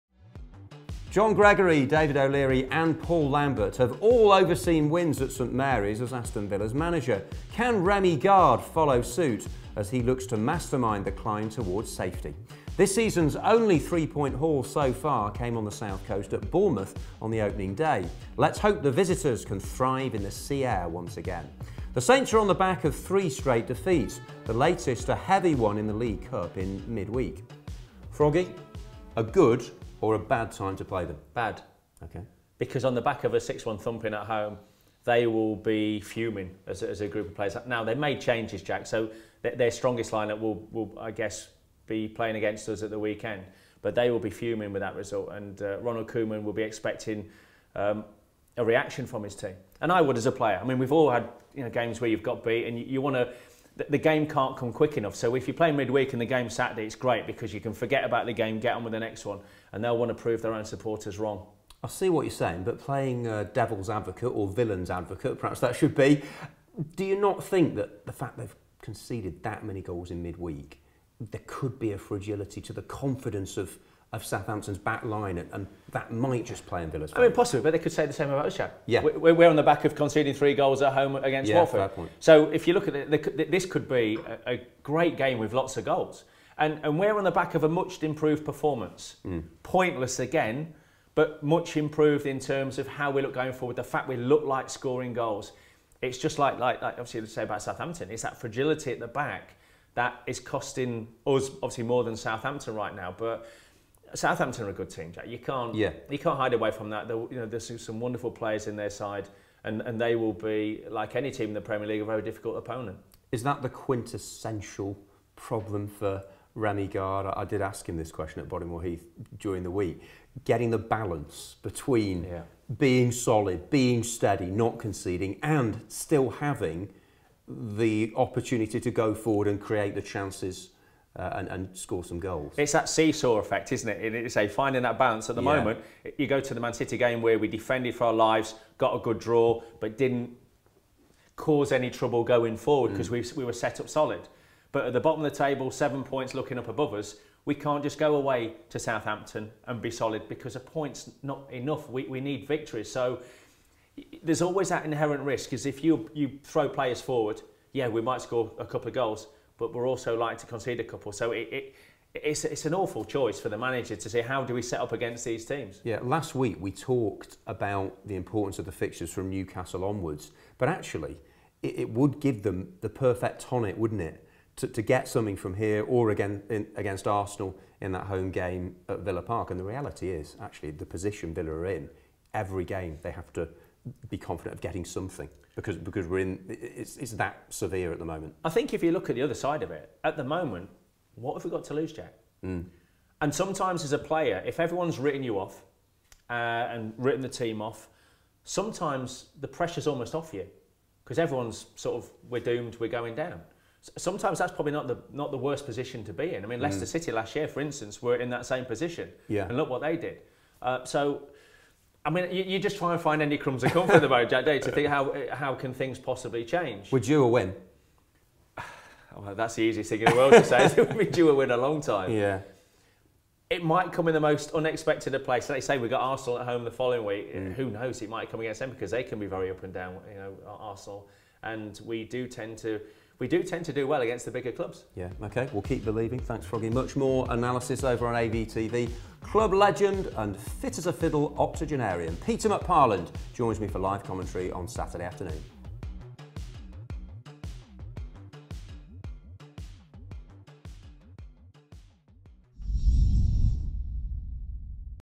Southampton pre-match show preview
A preview of what's on this week's pre-match show as regular pundit Steve Froggatt joins Jack Woodward in the studio to look ahead to our trip to Southampton tomorrow afternoon.